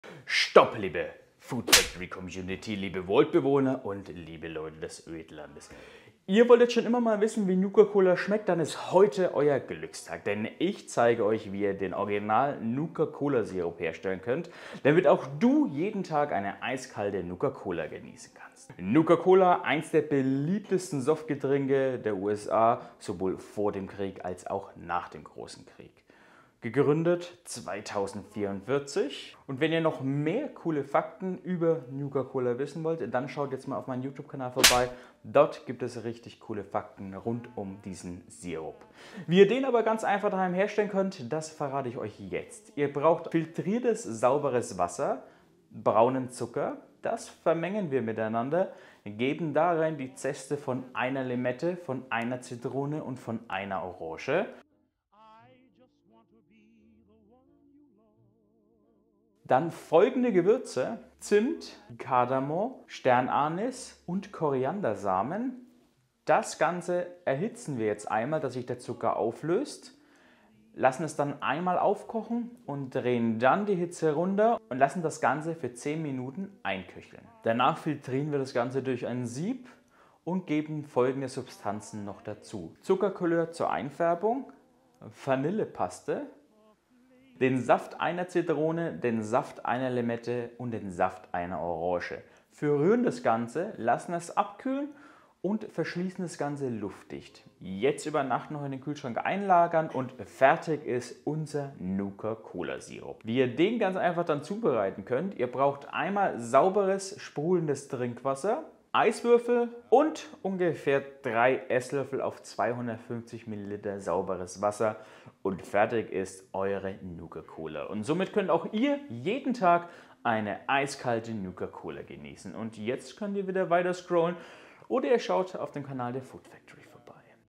Wie du eine eiskalte nuka cola machst | Short | Reel
Hallo liebe Food Factory Community,

Hast du Lust herauszufinden, wie erfrischend eine eiskalte Nuka Cola schmeckt? Dann haben wir heute Glück! Ich zeige dir, wie du den Sirup zu Hause selbst herstellen kannst, um jeden Tag ein eiskaltes Nuka Cola zu genießen.

Die Zutaten:
500 g brauen Zucker
250 ml Wasser
1 Zitrone
1 Orange
1 Limette
1 Stange Zimt
2 Sternanis
2 Kardamom Kapseln
6 Körner Koriander
1 TL Vanillepaste
1 TL Zuckerkulör

Die Zubereitung:
Schritt 01: Gib Wasser und Zucker in einen Topf und füge den Abrieb von Zitrone, Orange und Limette sowie die Gewürze hinzu. Erwärme alles leicht, bis sich der Zucker aufgelöst hat, dann lass es aufkochen und köchle für 10 Minuten bei mittlerer Hitze.

Schritt 02: Gieße den Sirup durch ein Sieb in ein verschließbares Gefäß. Füge den Saft von Zitrone, Orange und Limette hinzu und vermische alles. Gib Zuckerkulör und Vanillepaste dazu und rühre alles gut um. Verschließe das Gefäß luftdicht und lass es über Nacht im Kühlschrank ziehen.

Der Sirup ist bis zu 2 Wochen haltbar.

Schritt 03: Fülle Eiswürfel in ein Glas, gib 3 bis 5 Esslöffel Sirup dazu und fülle dann mit Sprudelwasser auf, um deine eiskalte Nuka Cola zu genießen.